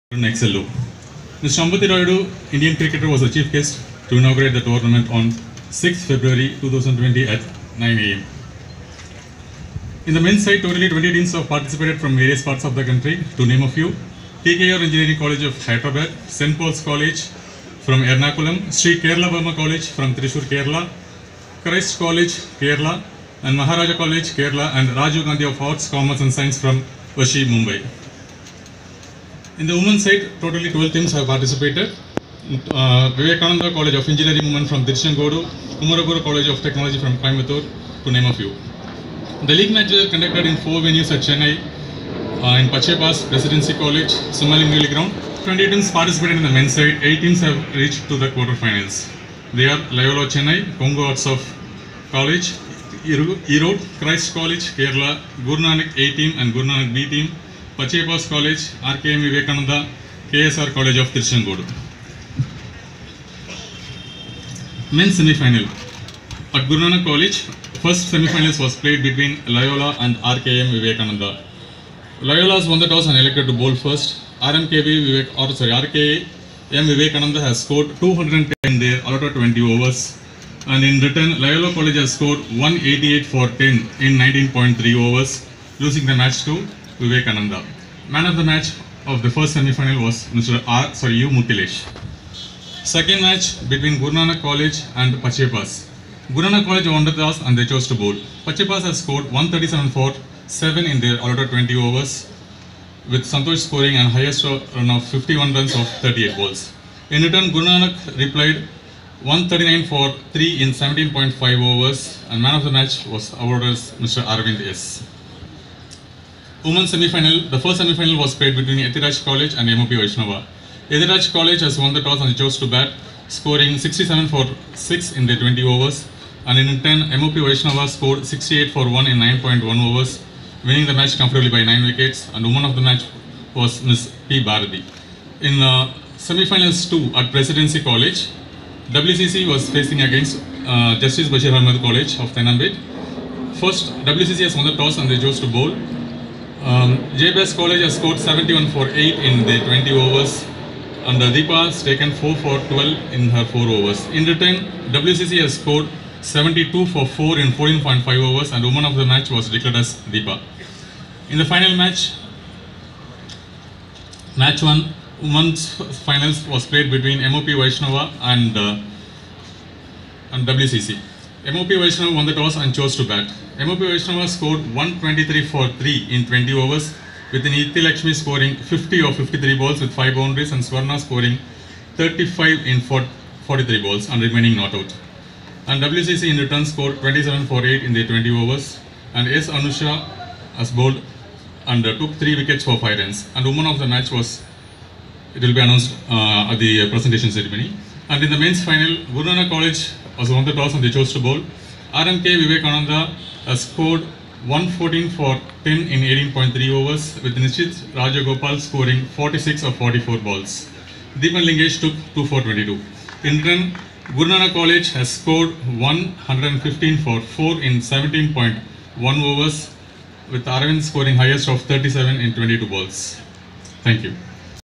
T20 inter Collegiate Cricket Tournament 2019-20 Hosted by Guru Nanak College, Chennai
W O M E N – Finals
Ground : Guru Nanak College Ground, Toss : MOP chose to bat
MOP Vaishnava 123 for three in 20 overs
R. Nithya Lakshmi 50 (53b, 5x4),
S. Swarna 35 not out
beat WCC 27 for eight 20 overs
S. Anusha three for five
Eve of the Final and Player of Tournament : R. Nithya Lakshmi
(MOP)
M E N – Finals
Ground : Guru Nanak College Ground, Toss : GNC chose to Field
RKM Vivekananda 114 in 18.3 overs
Nidhish S. Rajagopal 46 (44b, 5x4, 2x6),
K. Deeban Lingesh two for 22,
S. Aravind two for 15,
Vignesh Yadav two for 17
lost to Guru Nanak 115 for four in 17.1 overs
S. Aravind 37 (22b, 4x4, 2x6)
Awards:
Player of the final : Vignesh Yadav (GNC)
Player of the Tournament : S. Aravind (GNC)
Best Batsman : Nidhish S. Rajagopal (Viveka)
Best Bowler : T.D. Lokesh Raj (GNC)
Best All Rounder : U. Mukilesh (Viveka)
Best Fielder : Bharath Hariharan (GNC)
Best Wicketkeeper : K. Sivakumar
Best Cricketer : K. Deeban Lingesh (GNC)
Mr. L. Balaji gave away the trophies and prizes